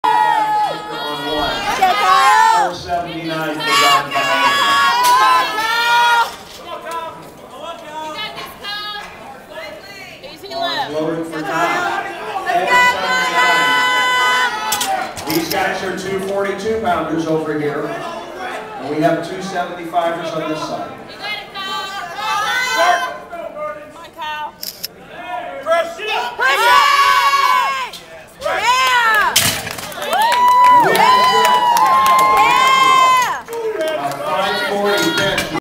Kyle Ramsey Bench press 540 Lbs at Collegiate Nationals 2010
Kyle took 2nd place over all in his division in Orlando Fl. at the "2010, 28th National Collegiate Powerlifting Championships"